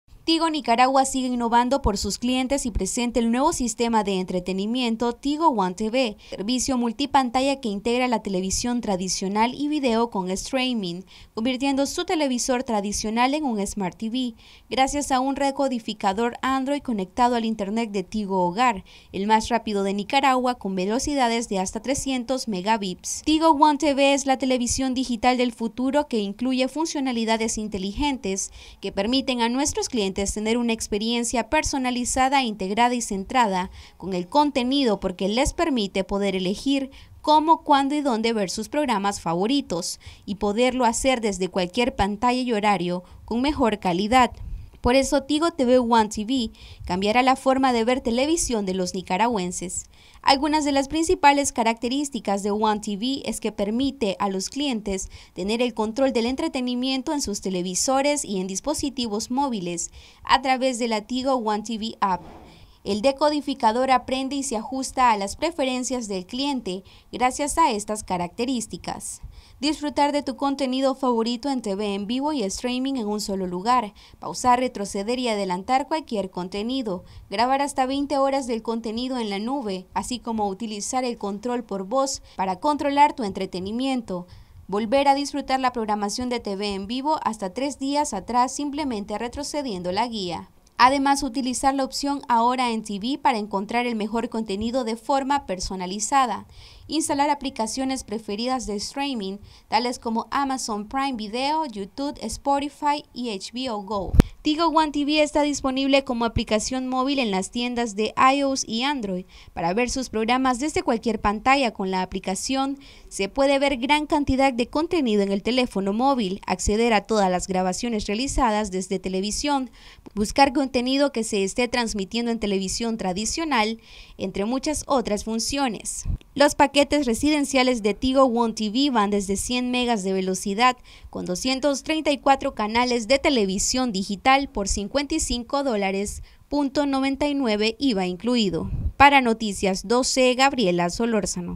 Tigo Nicaragua presenta nuevo sistema de entretenimiento para sus clientes
Noticias12Nicaragua Tigo Nicaragua presenta nuevo sistema de entretenimiento para sus clientes.